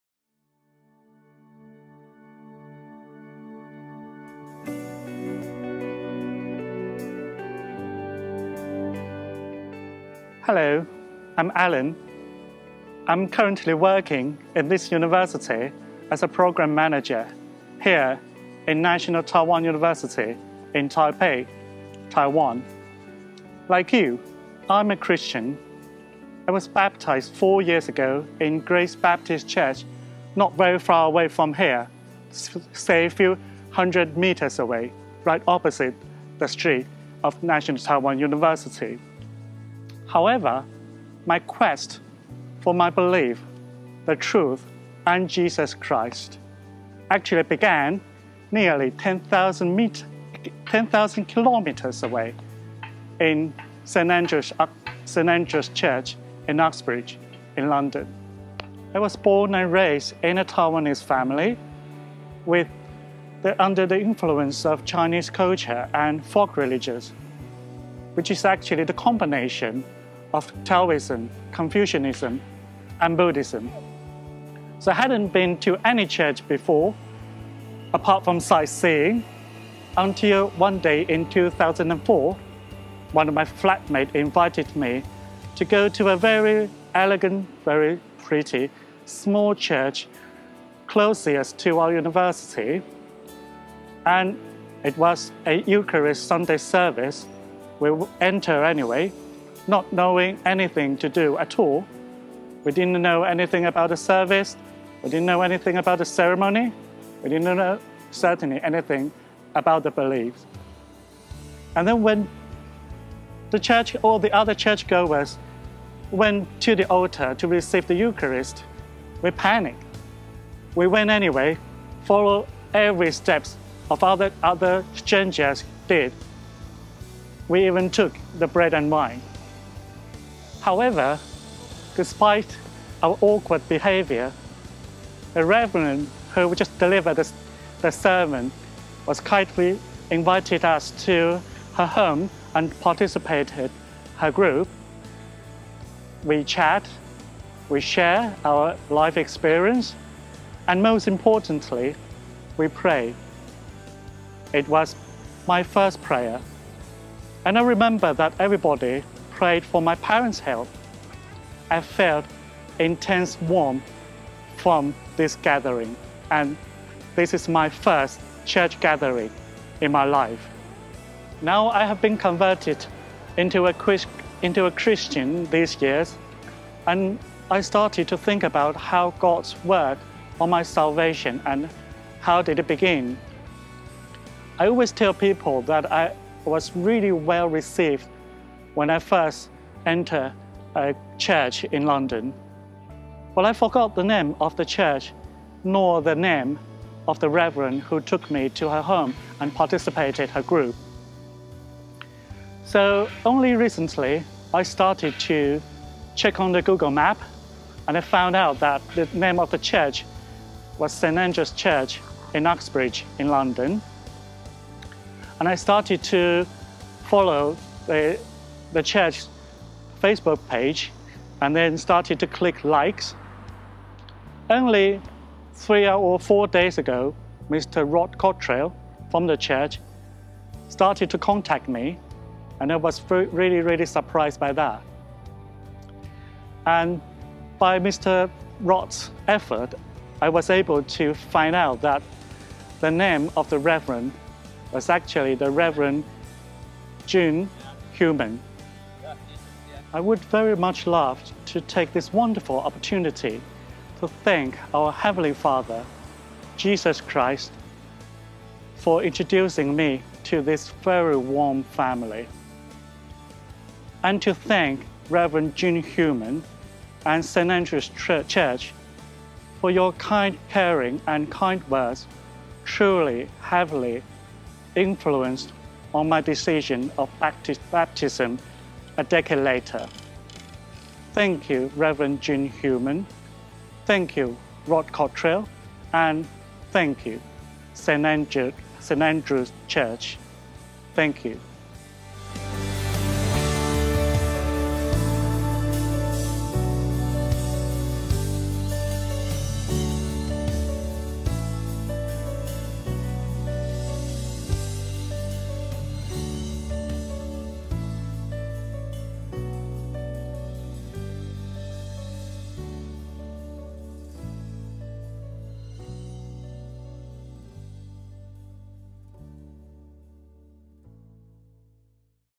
From Atheism to Baptism - a 10 year Journey
An inspirational testimony from Alan